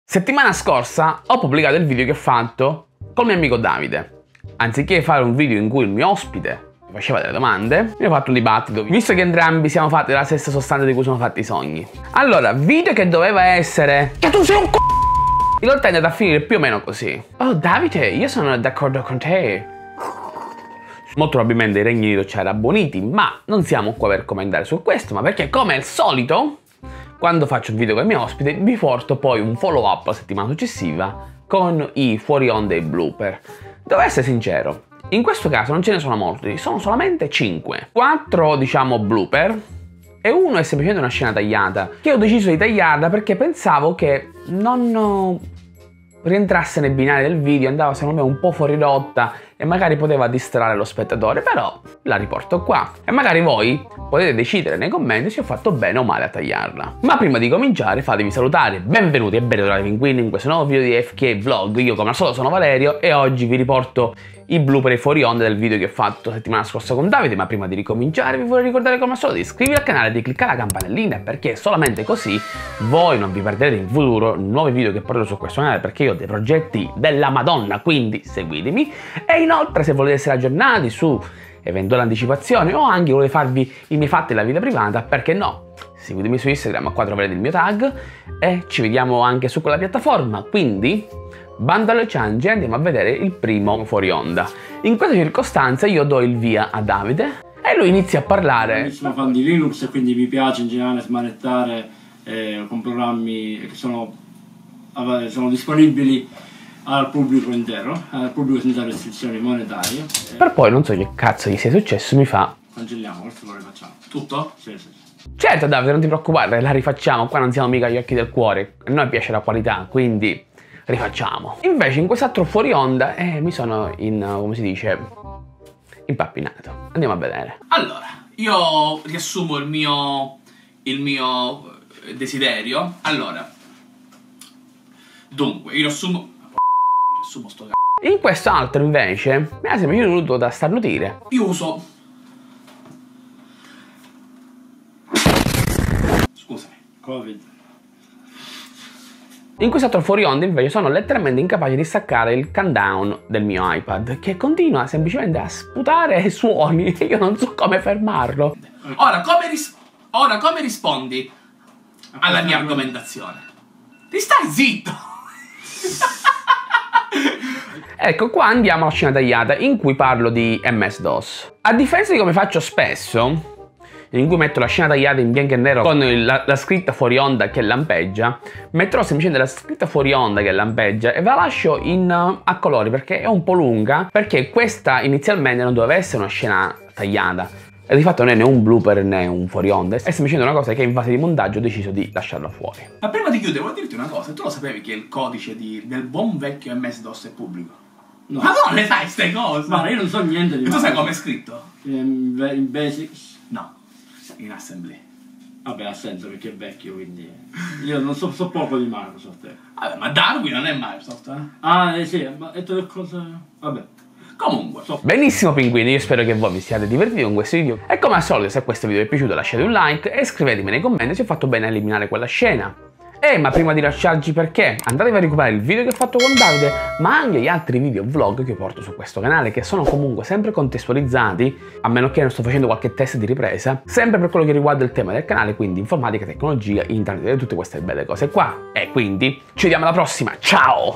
SCENE TAGLIATE prese dal dibattito del video 'OPEN vs CLOSED SOURCE - AFK VLOG
Anche questa settimana vi porto i fuorionda, blooper, e scene tagliate presi dal video che ho fatto con il mio amico Davide. Qualora ve lo siate persi, recuperatelo qua

In questo video, vi mostro una scena tagliata dove parlo di MS-DOS che, alla fine, ho deciso di farvela vedere in questo video.

Video VL-22-018